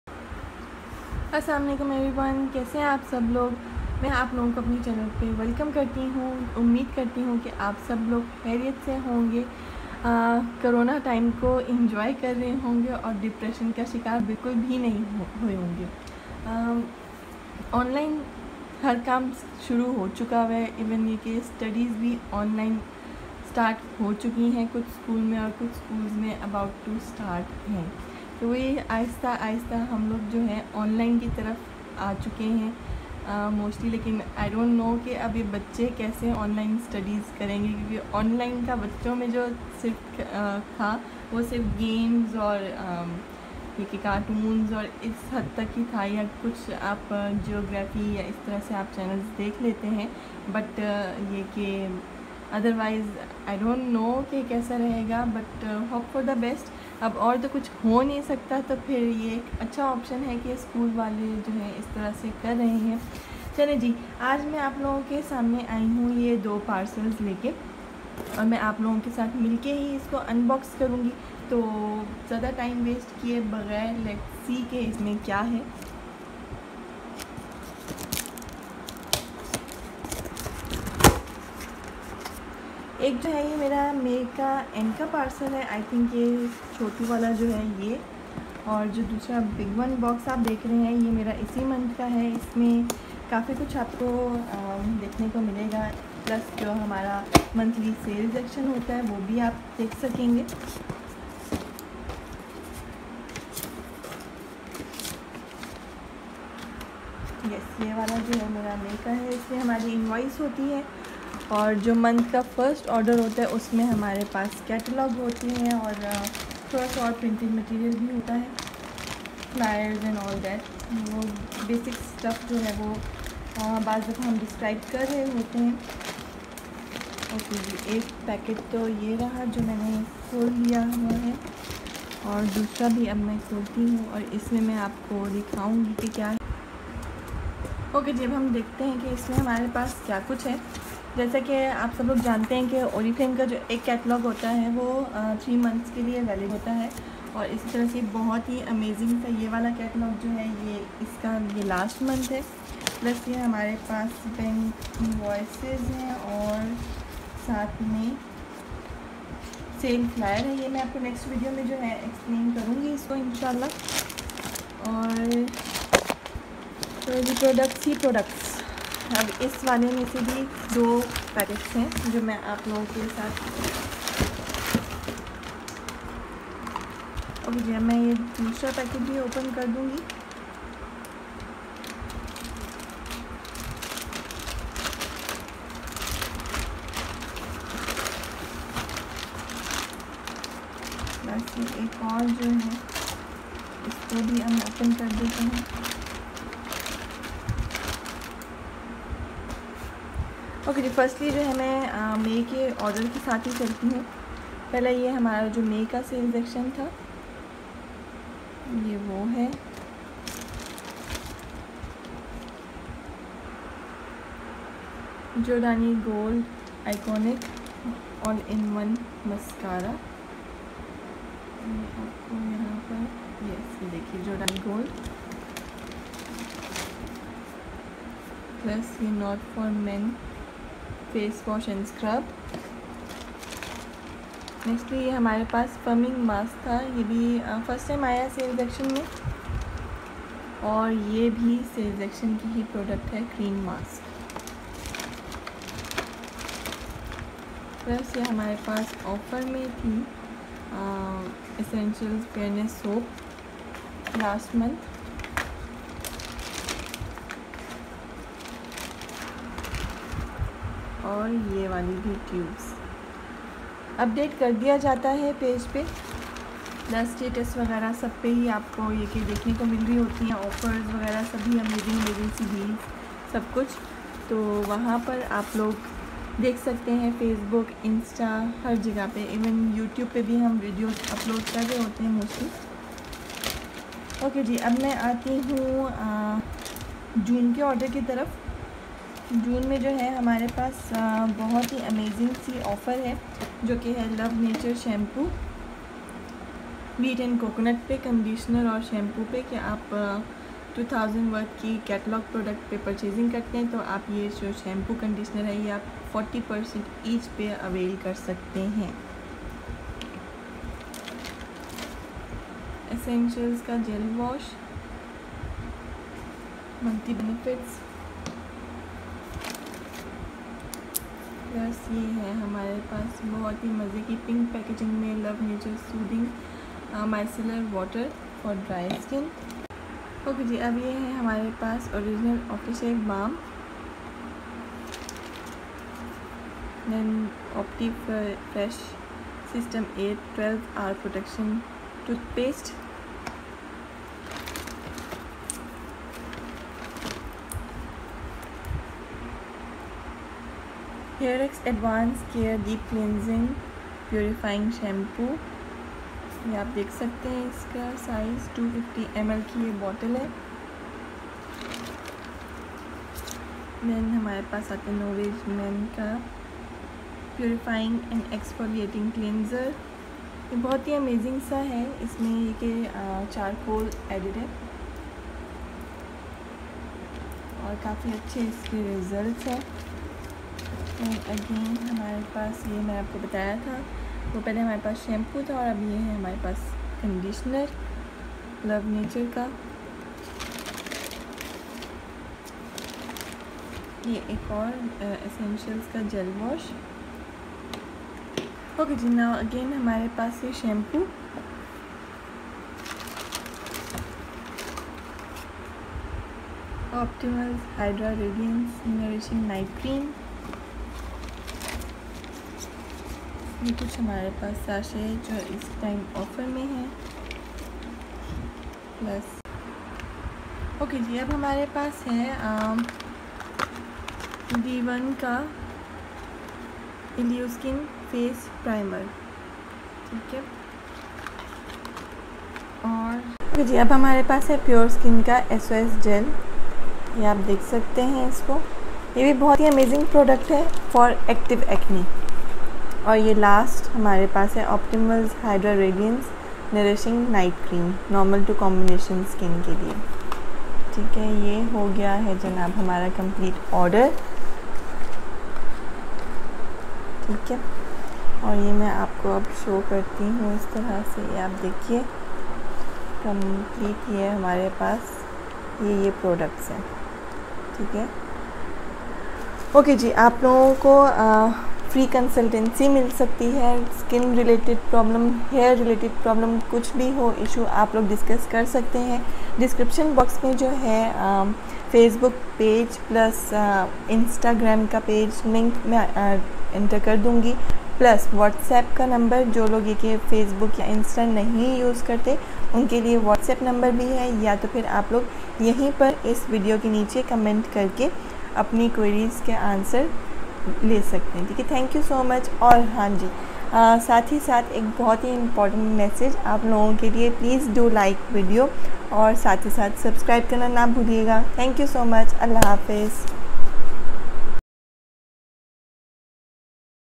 Throwback| May / June 2020| Oriflame | Customer’s Order|Personal Order| Taibaofficial
In this video,I am sharing the Customer order & Personal Order of May/ June ,in Urdu/Hindi.Throwback few months back.

 •  Current Catalogue